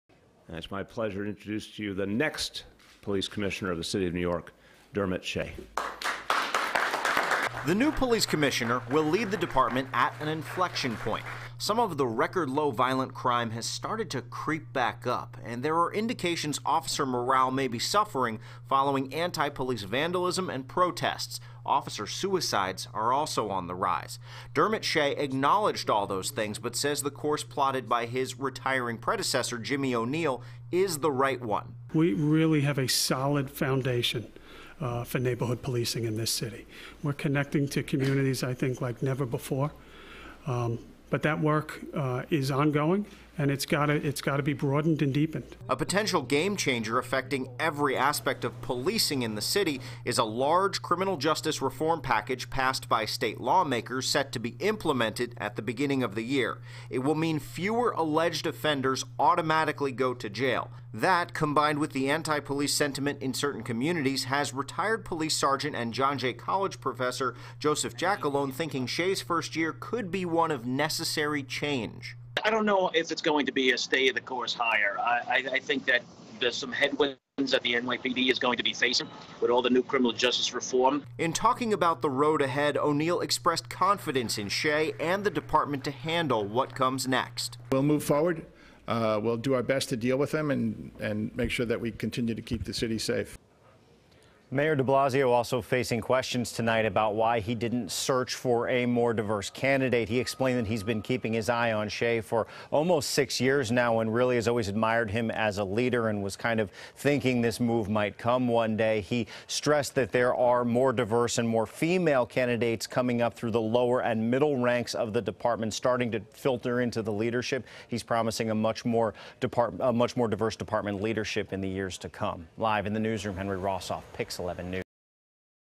NYPD Commissioner James O`Neill resigns
New York City Police Commissioner James O`Neill is stepping down from the force, Mayor Bill de Blasio announced at a news conference Monday afternoon.